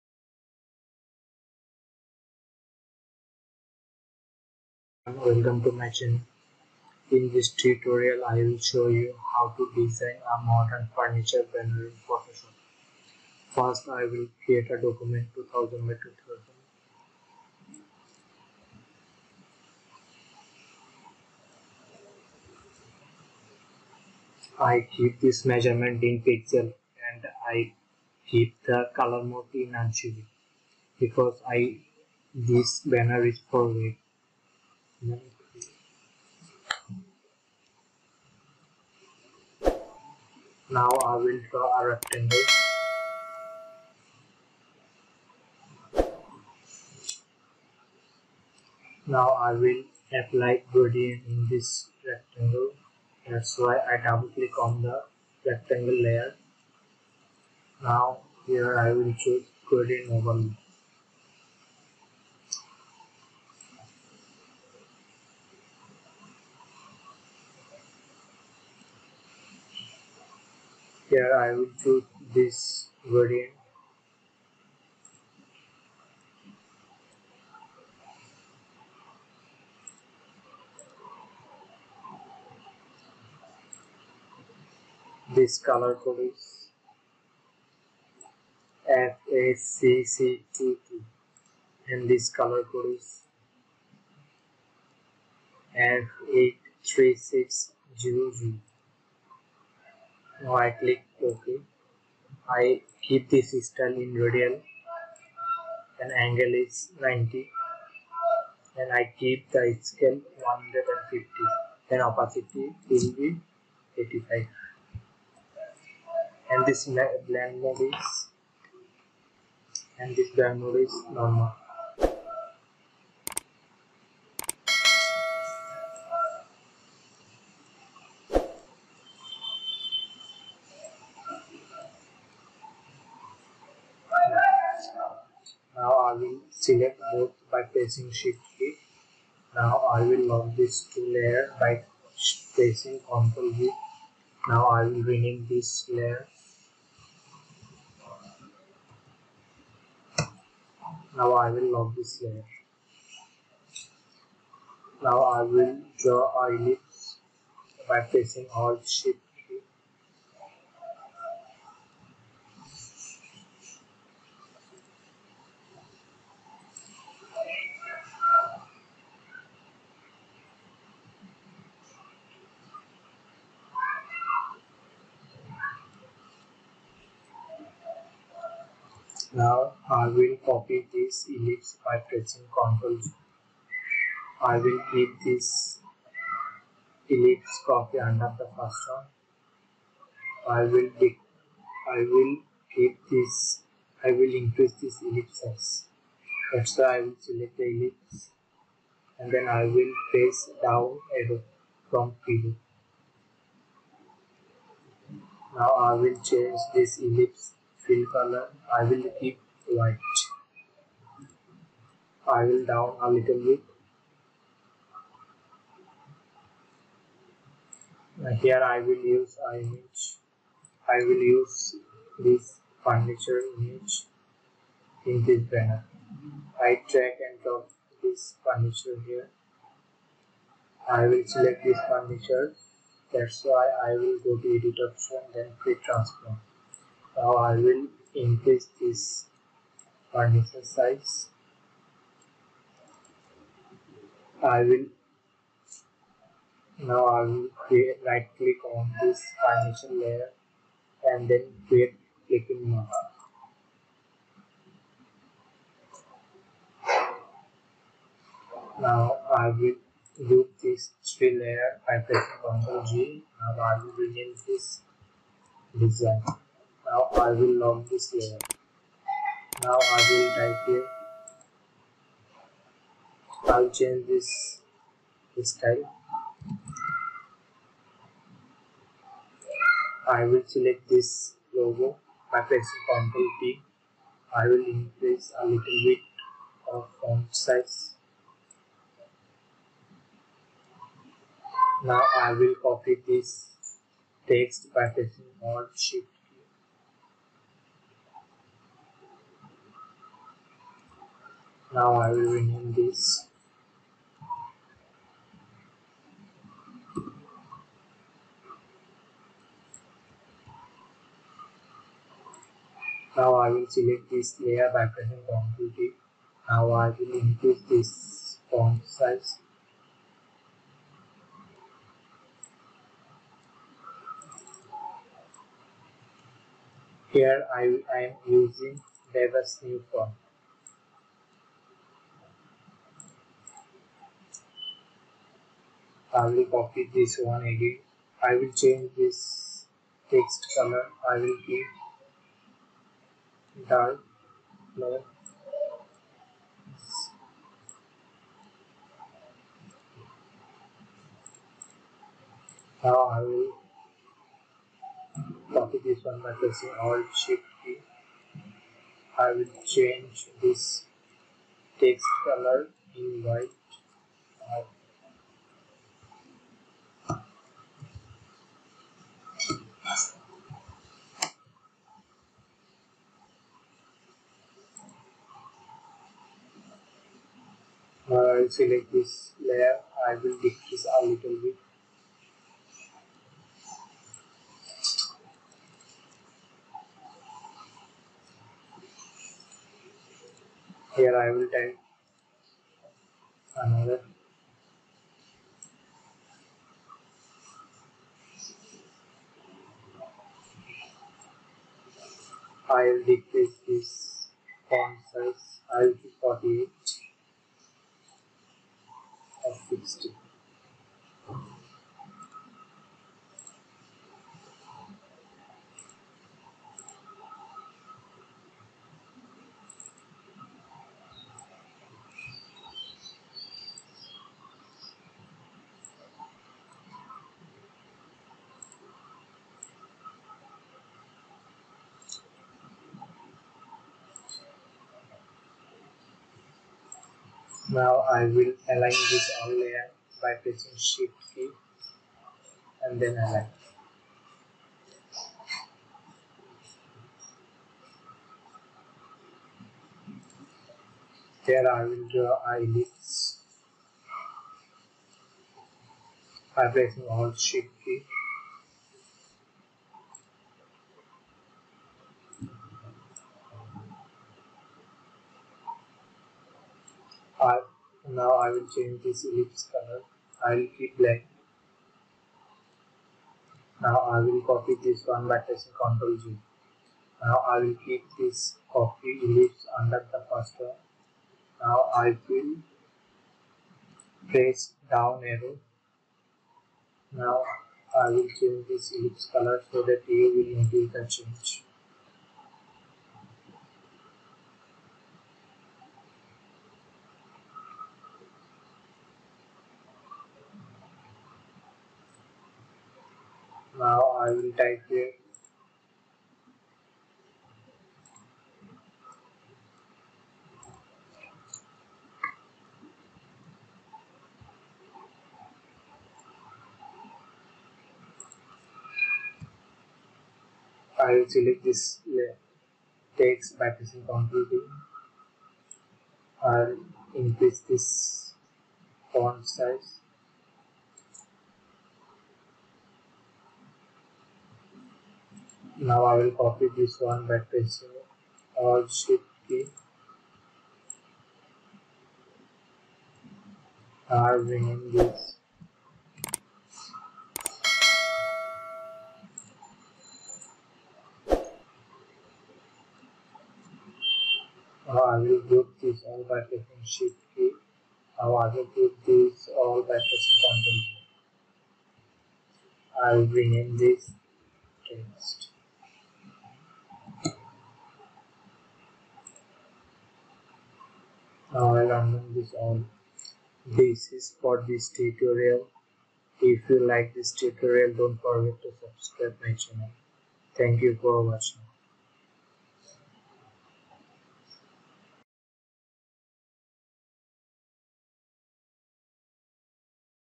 Modern furniture banner design in photoshop | social media banner design | photoshop tutorial
Today I am going to show you how to design Clean and Modern Furniture Banner Design in Photoshop in a easy way. After watching this tutorial you will able to design any kind of banner in Photoshop Easily and Quickly.